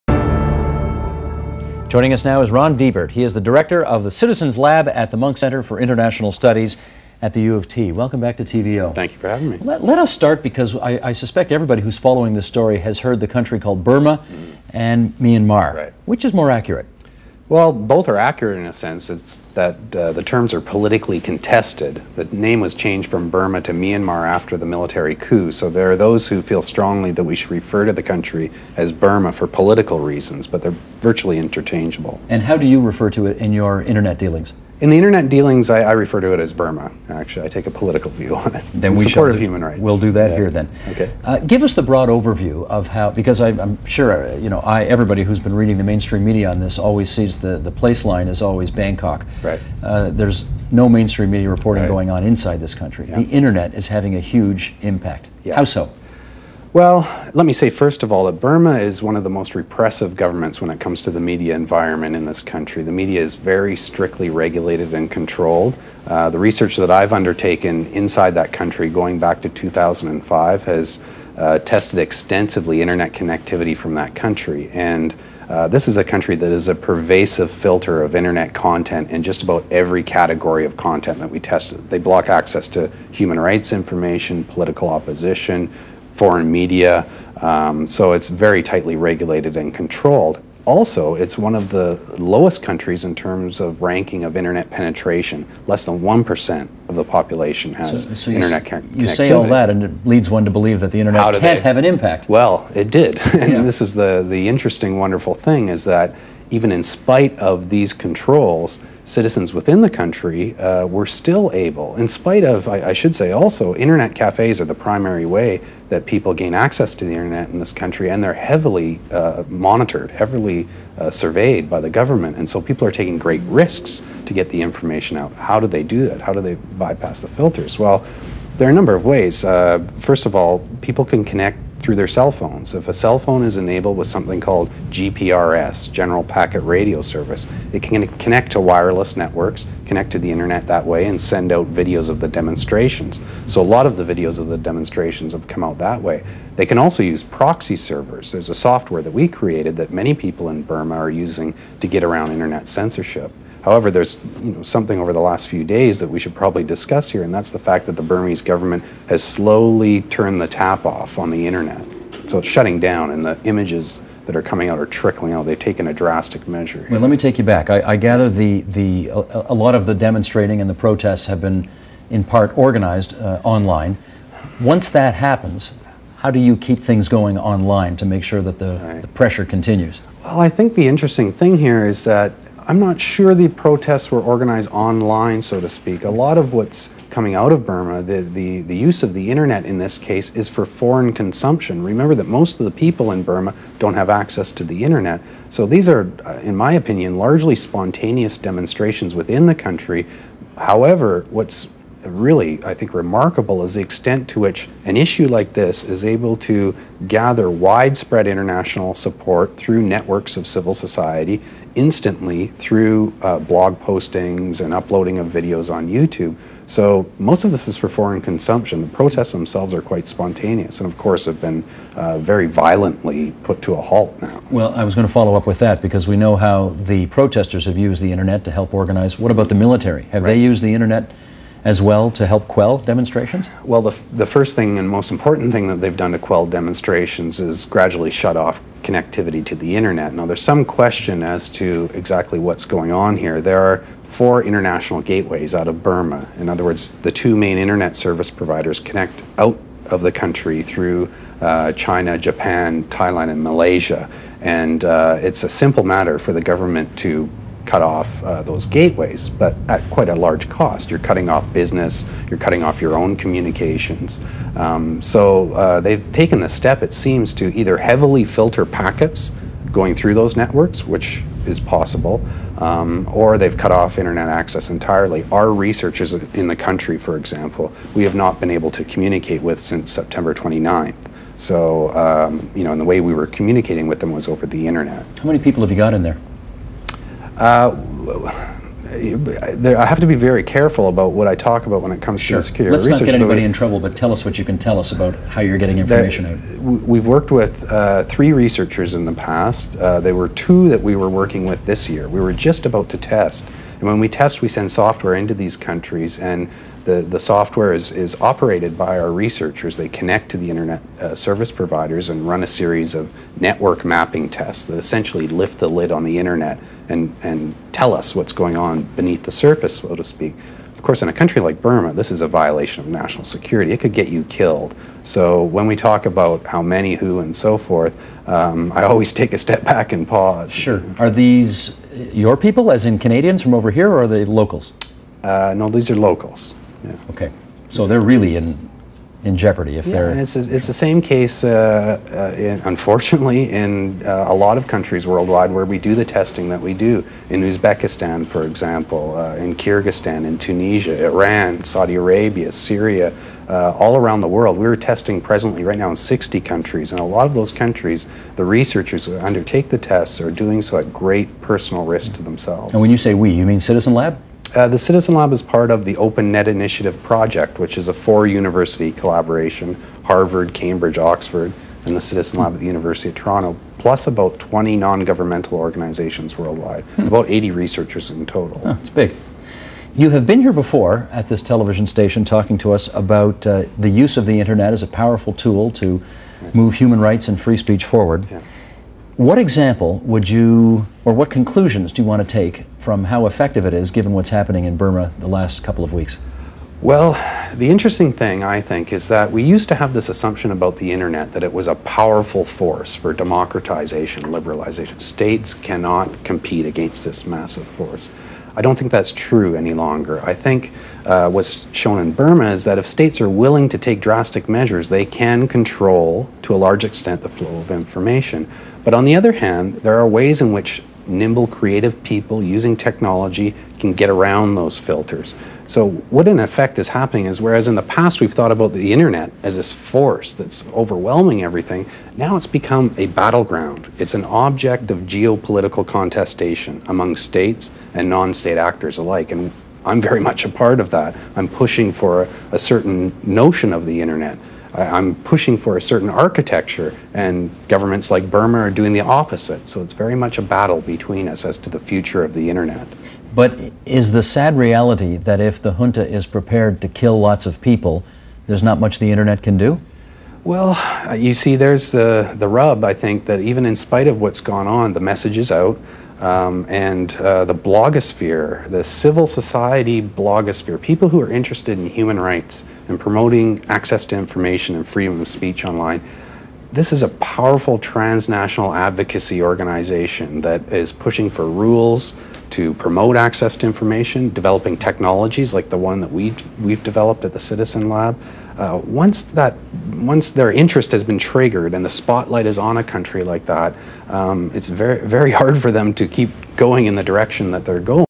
Ron Deibert on TVO - Part 1
Ron speaks about the situation in Burma, the Open Net Initiative, and his shirt.